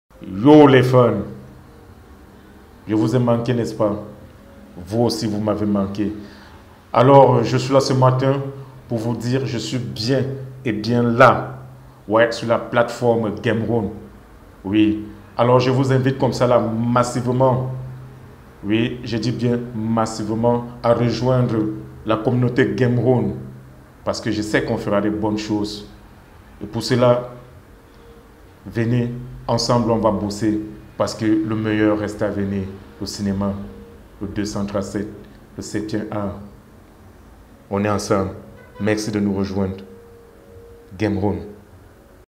Le Grand Cool Black et Gameroon Entertainment
Cool Black annonce qu'il appartient à la communauté de Gameroon Entertainment et vous invite à l'y rejoindre.